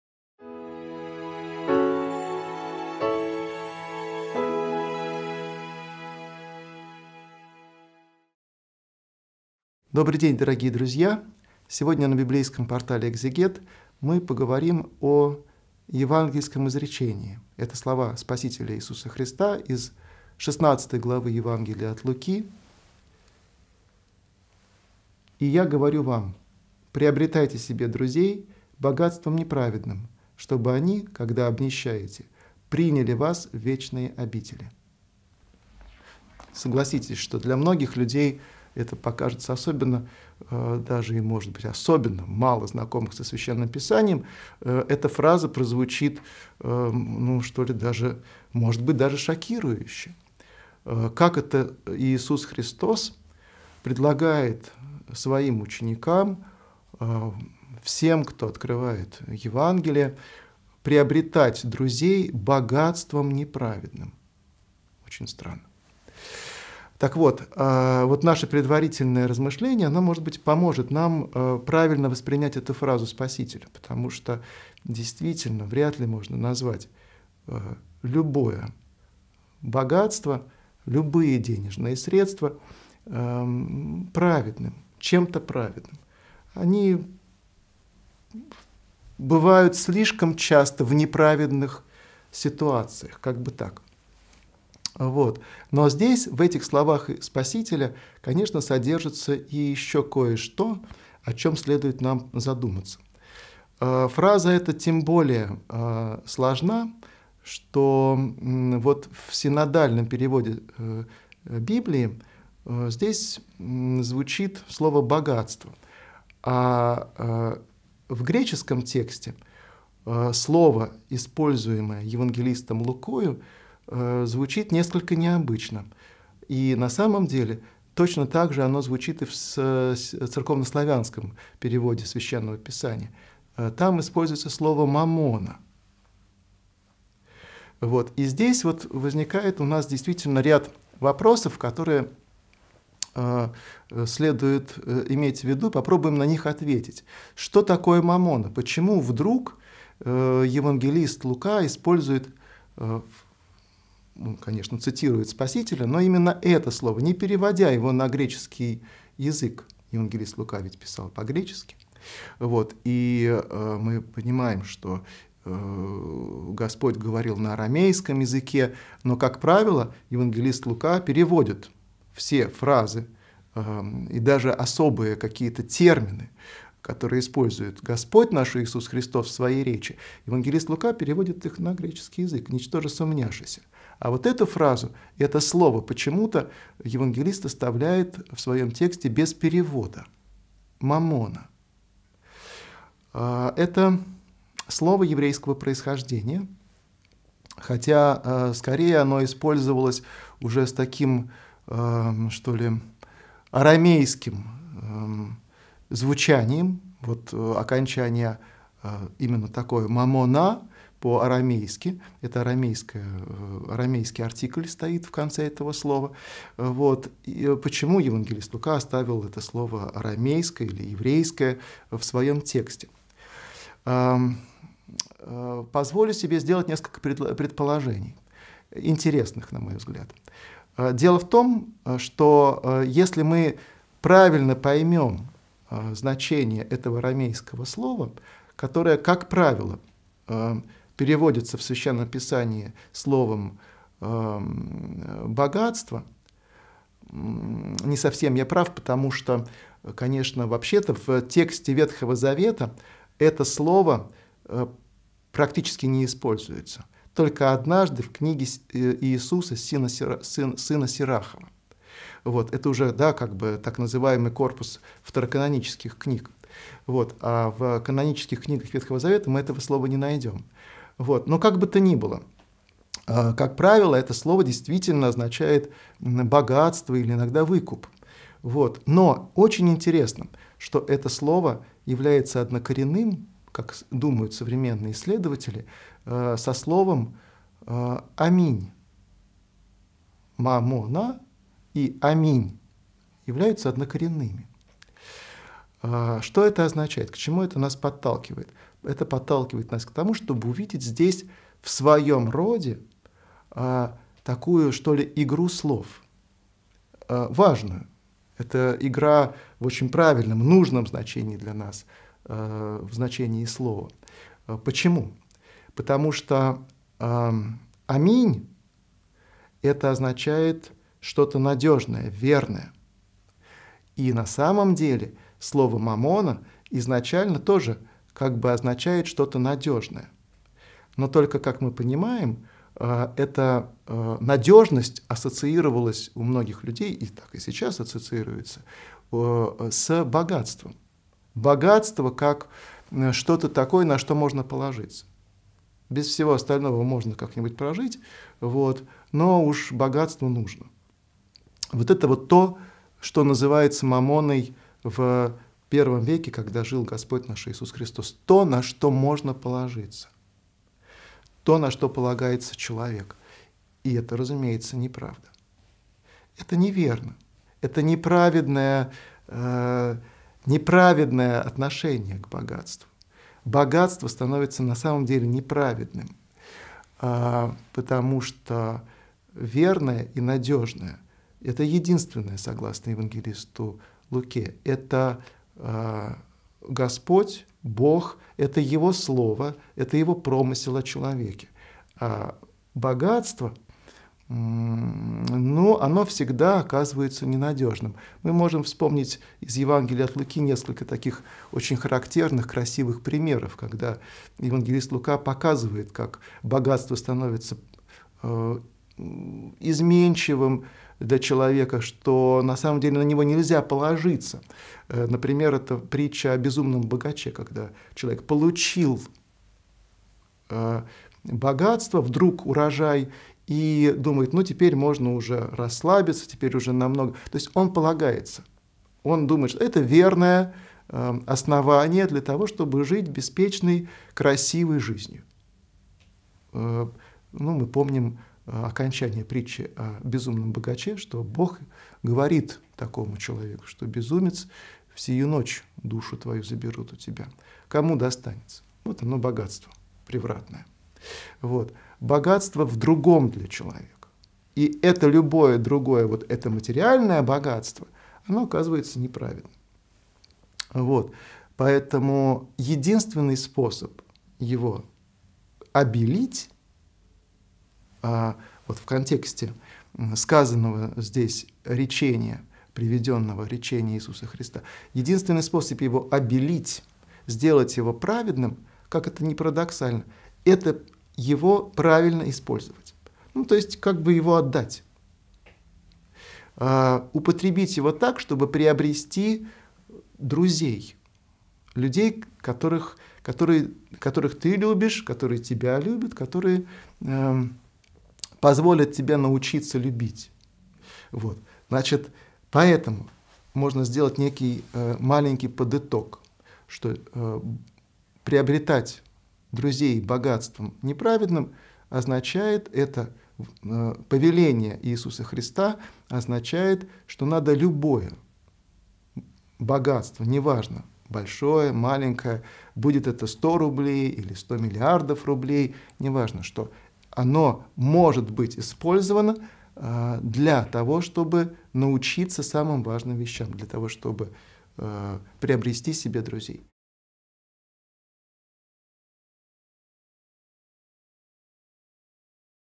"Приобретайте себе друзей богатством неправедным" (Лк. 16:9). Протоиерей Дионисий Лобов. Толкование
В сегодняшнем видео Протоиерей Дионисий Лобов прокомментирует стих "приобретайте себе друзей богатством неправедным" из Евангелия от Луки 16 главы, 9 стиха и коснется следующих тем:

🔸О словах Иисуса Христа из Евангелия от Луки
🔸О том, что эта фраза звучит довольно шокирующе
🔸О том, что любое богатство сложно назвать чем-то праведным
🔸О том, как в греческом тексте звучит слово богатство?
🔸Что такое маммона?
🔸Почему Евангелист Лука оставляет это слово без перевода?
🔸О происхождении этого слова
🔸Об игре однокоренных слов
🔸О том на что полагается человек
🔸Что единственное верно и надежно?
🔸Об изменчивости богатства
🔸О правильном использовании богатства